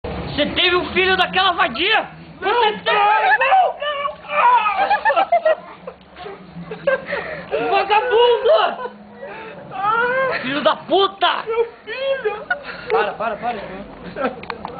Marcito grávido
Marcito grávido perde o filho pelas mãos de Tissão Cuzão